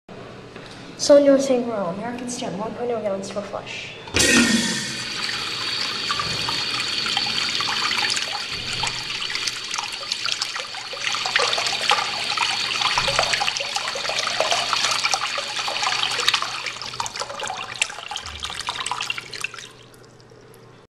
2986: American Standard Washbrook Urinal 15
This urinal is located in the men's room on the 1st floor of the visitor center at the Hiram M. Chittenden Locks in Seattle, WA.
This video was filmed on July 7, 2011.